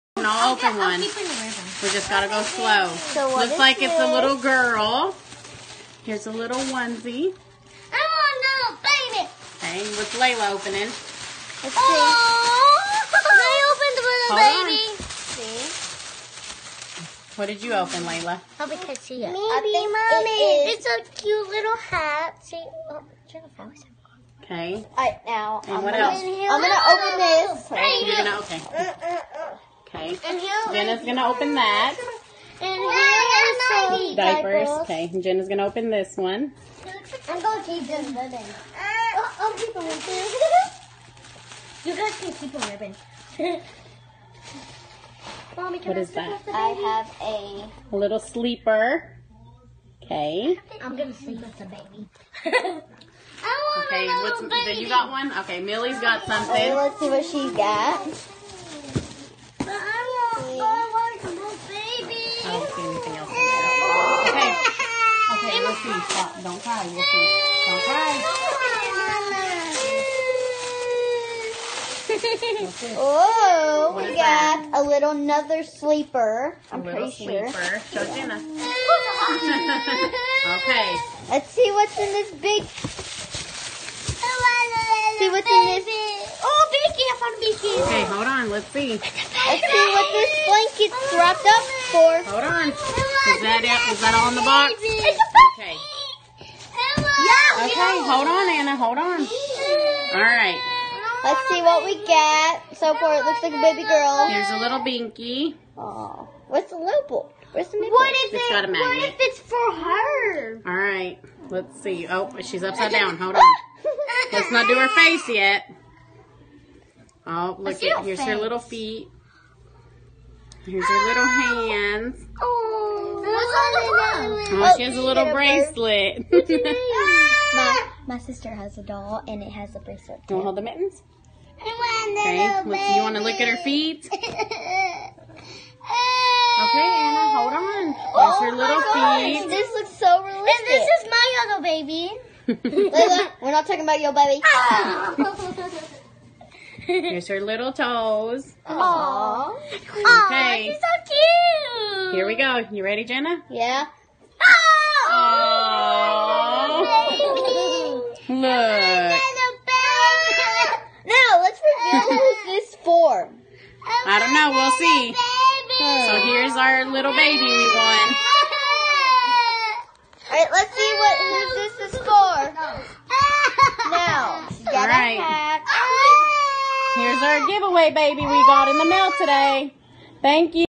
Part 2 of our REBORN GIVEAWAY box opening for Jan 31st
This is the part 2 of the box opening of our GIVEAWAY reborn that was won Jan 31 st We have a new GIVEAWAY up now come join us and enter in on Feb 28th Reborn GIVEAWAY .We would love to have you come and follow us in our journey.